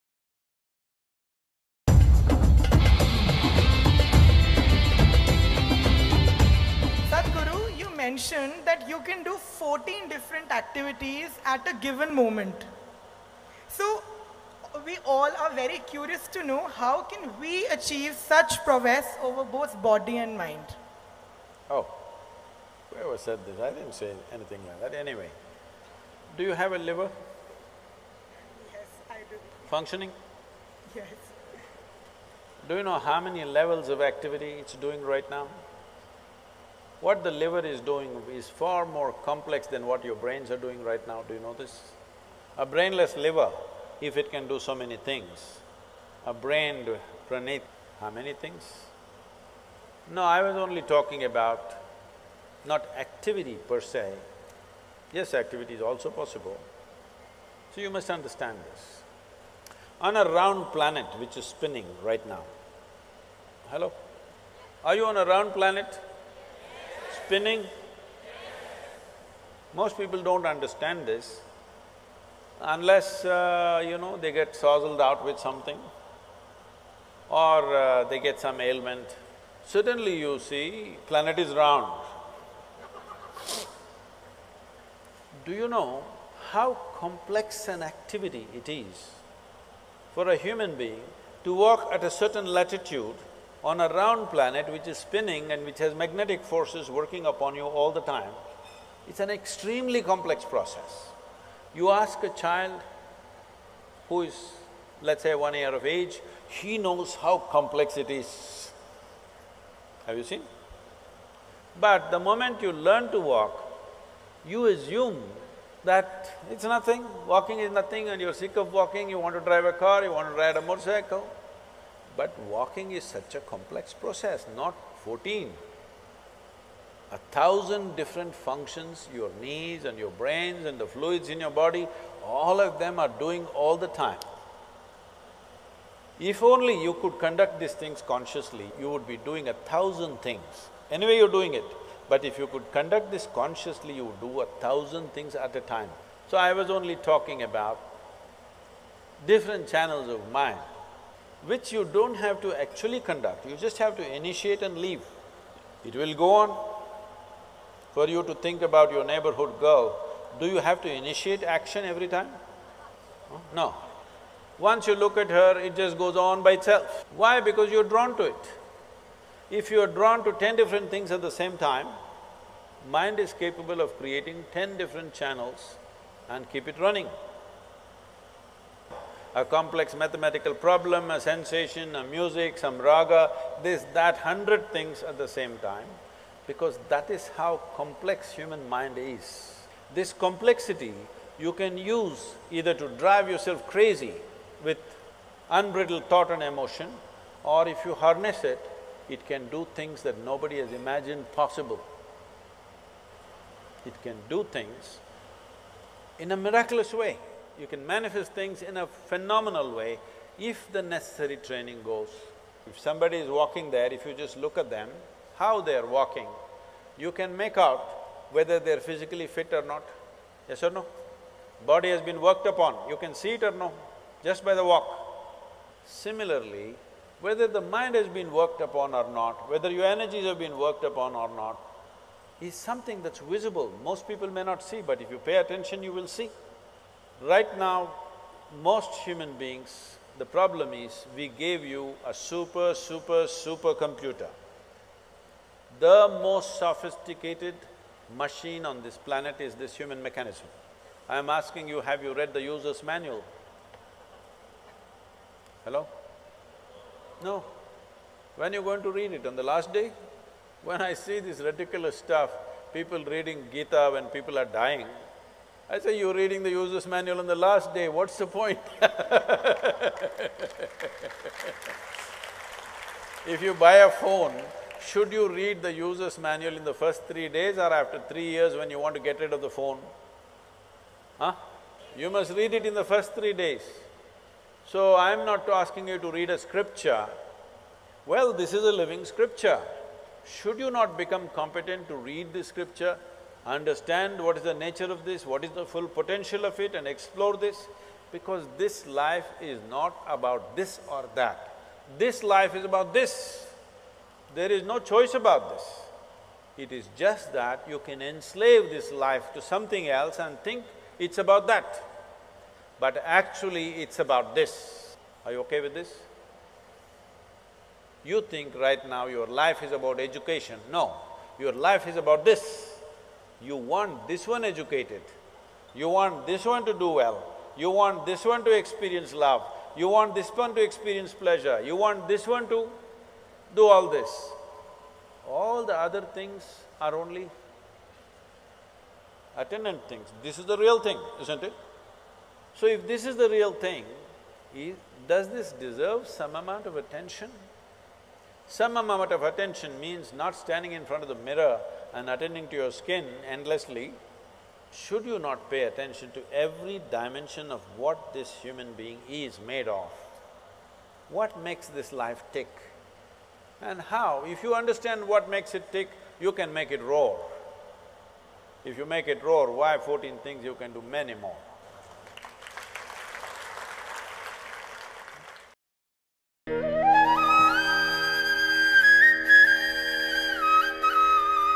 心智的惊人力量 | Sadhguru 萨古鲁
一个人能同时做多少事？萨古鲁谈到同时做多个任务所需的条件。thingsyoushouldknow